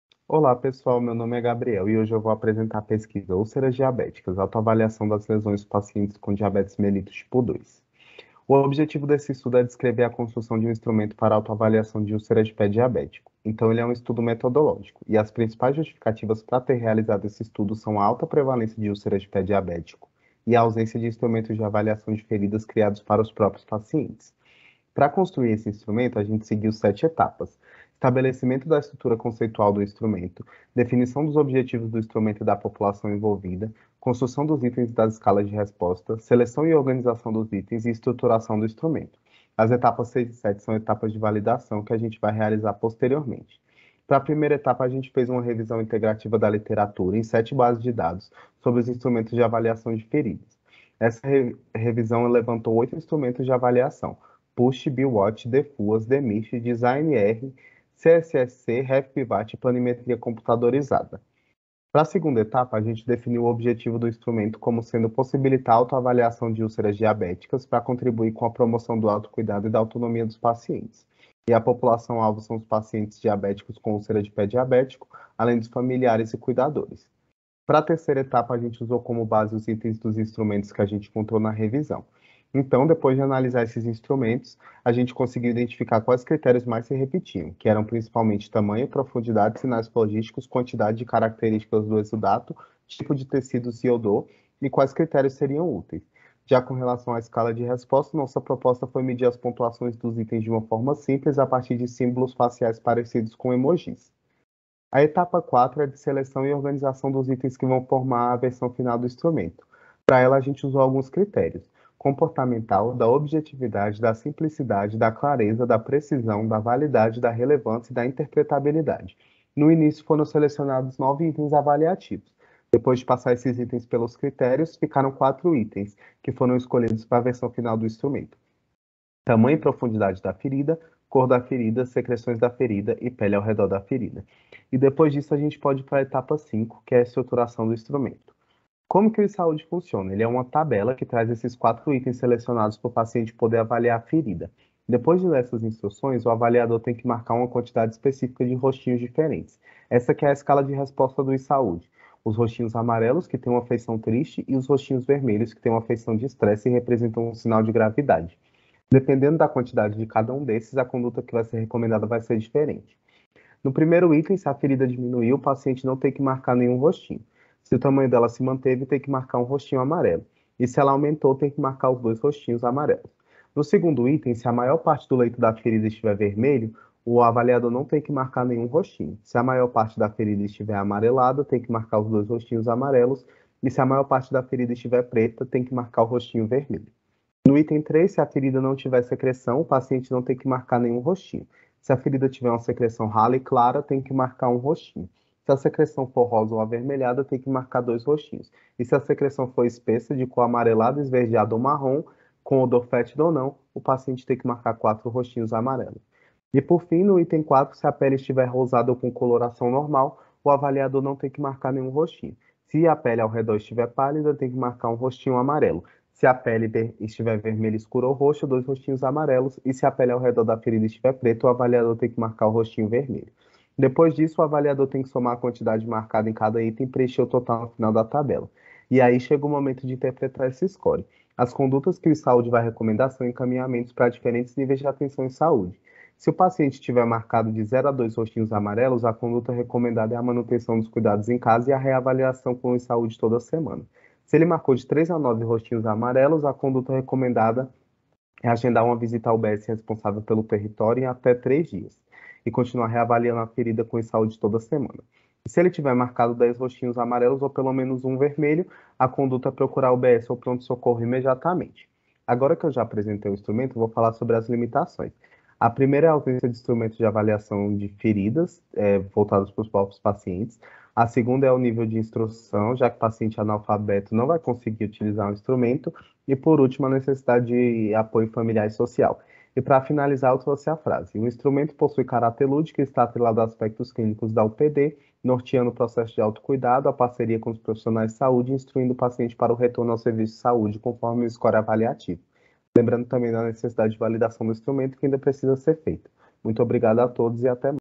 Construção de um instrumento de autoavaliação de úlceras diabéticas por pacientes com diabetes melli
Construção de um instrumento de autoavaliação de úlceras diabéticas por pacientes com diabetes mellitus tipo 2
Grande Área: Saúde e Vida
Aluno: GABRIEL ROCHA MARCELINO
Orientador: PRISCILLA ROBERTA SILVA ROCHA
Palavras-chaves: Autoavaliação. Pé Diabético. Complicações do Diabetes.